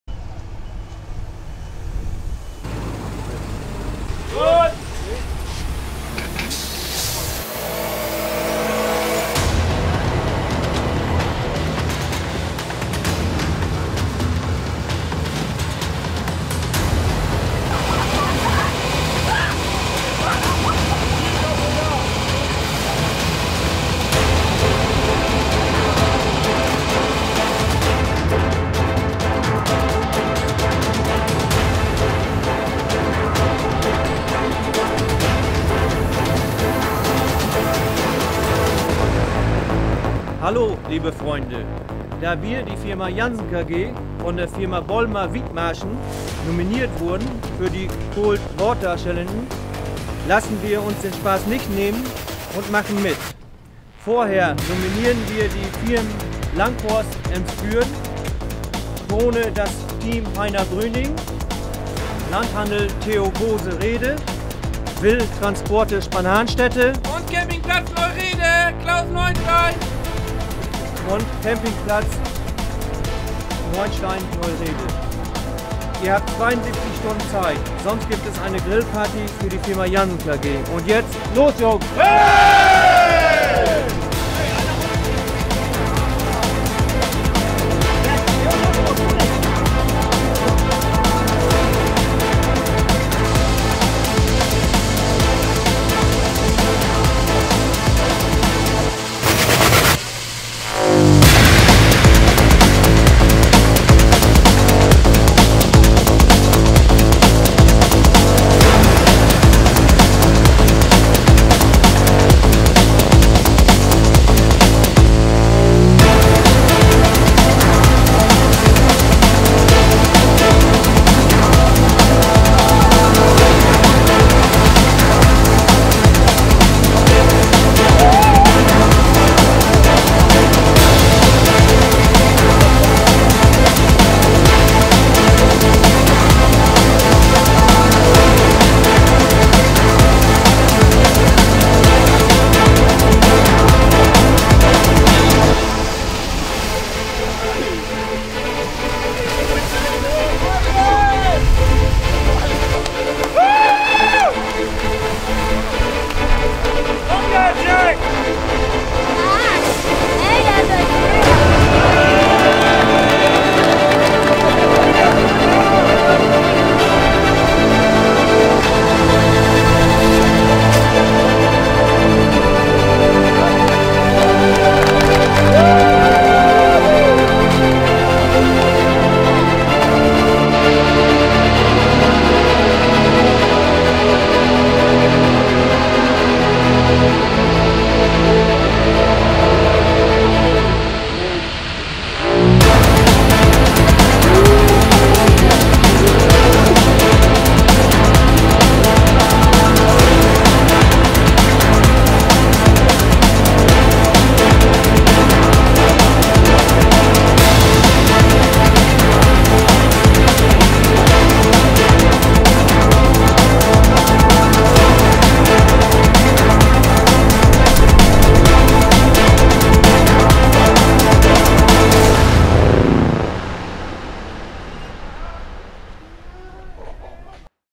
Janssen KG - Cool Water Challenge 2014
Für die vielen freundlichen Nominierungen die wir in den letzten Tagen bekommen haben, seht ihr hier unseren kleinen Beitrag zur Challenge.

Mit 20.000 Liter Wasser eine 15 m lange Maissilage-Rutsche und mit 10 Tuben Spülmittel gabe es einen feucht-fröhlichen Spaß.

Und schön aufpassen, wenn wir alles nominiert haben!

Euer Team Janssen KG